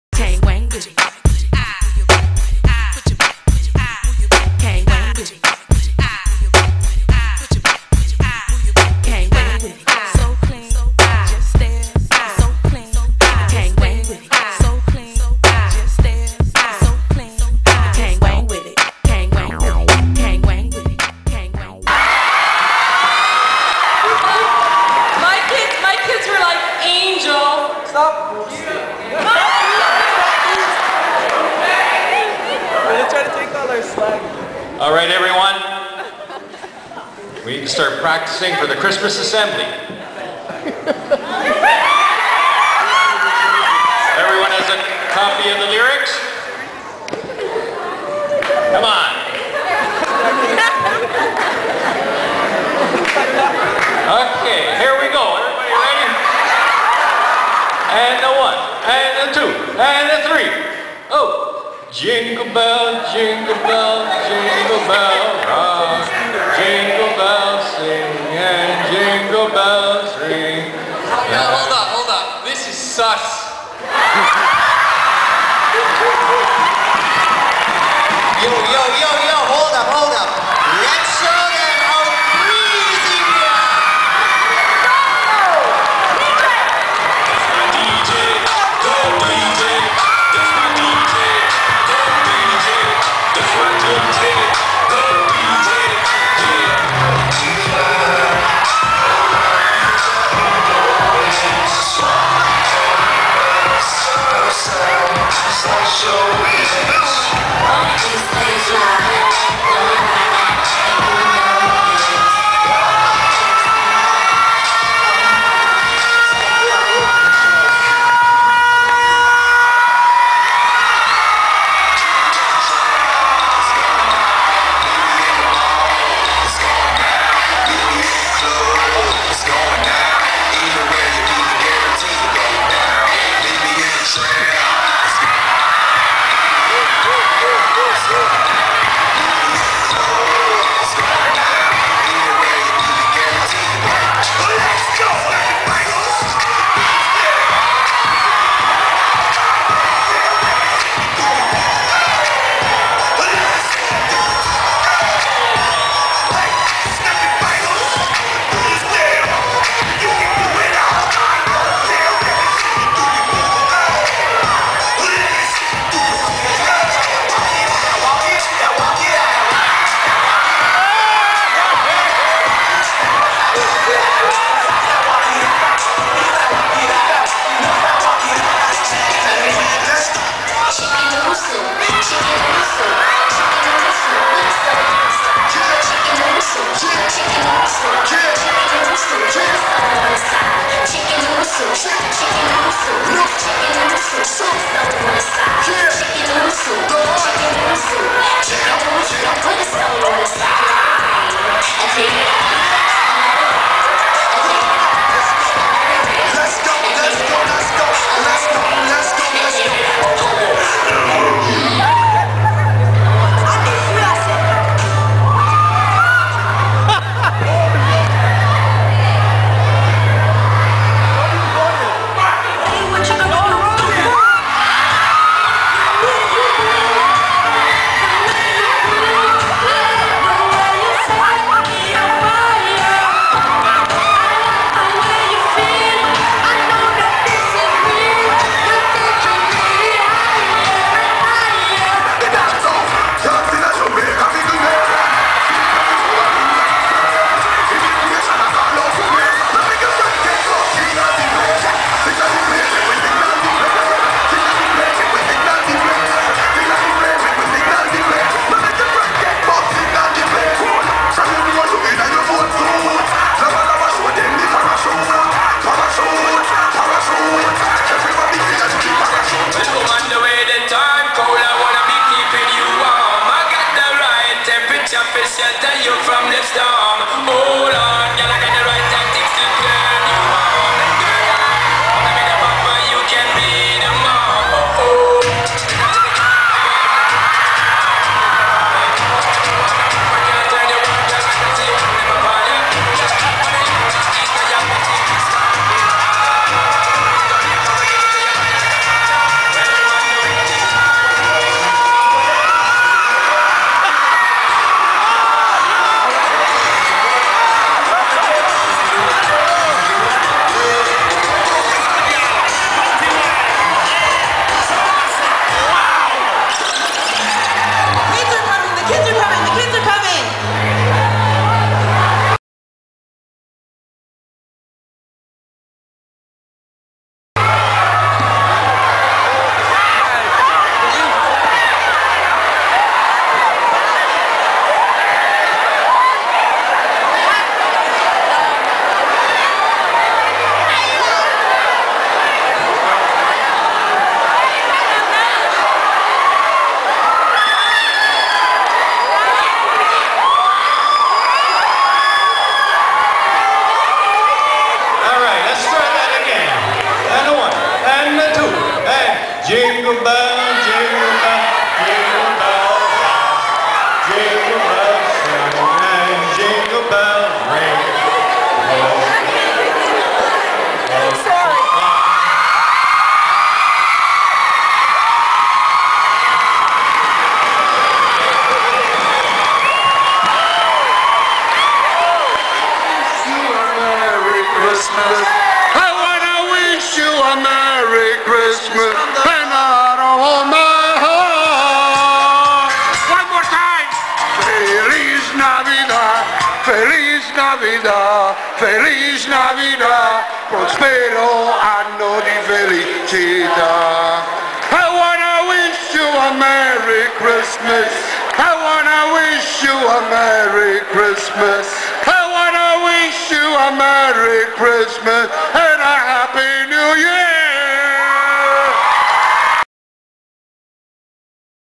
Father Henry Carr Christmas Assembly 06'
A MUST WATCH. cant even describe... highschool teachers at the christmas assembly dancing and singing. a classic. so what your teacher can dance better then you.. your school is in the ghetto